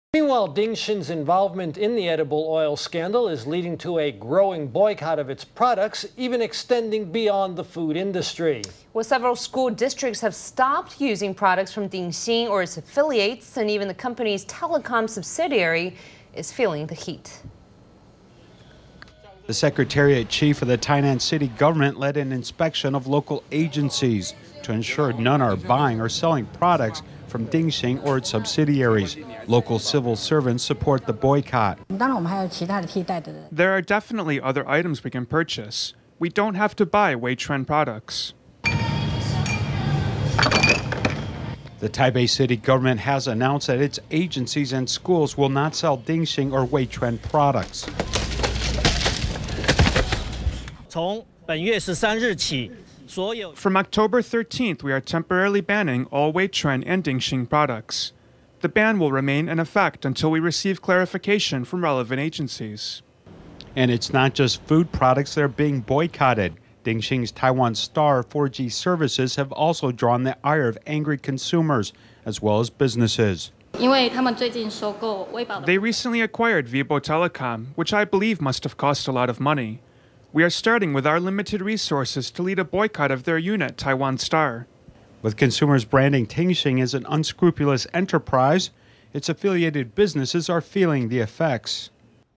Consumer and municipal groups boycott Ting Hsin and Wei Chuan products
Ting Hsin’s involvement in the edible oil scandal is leading to a growing boycott of its products. The Secretariat chief of the Tainan City government led an inspection of local agencies to ensure none are buying or selling products from Ting Hsin or its subsidiaries. Local civil servants support the boycott. Tainan City Hall EmployeeThere are definitely other items we can purchase. We don’t have to buy Wei Chuan products.The Taipei City government has announced that its agencies and schools will not sell Ting Hsin or Wei Chuan products.Chen Shun-hoTaipei Department of EducationFrom Oct. 13 we temporarily banned all Wei Chuan and Ting Hsin products. The ban will remain in effect until we receive clarification from relevant agencies.  And it’s not just food products that are being boycotted. Ting Hsin’s Taiwan Star 4G services have also drawn the ire of angry consumers as well as businesses.Yilan Telecom OperatorThey recently acquired Vibo Telecom, which I believe must have cost a lot of money. We are starting with our limited resources to lead a boycott of their Taiwan Star unit.With consumers branding Ting Hsin as an unscrupulous enterprise, its affiliated businesses are feeling the effects.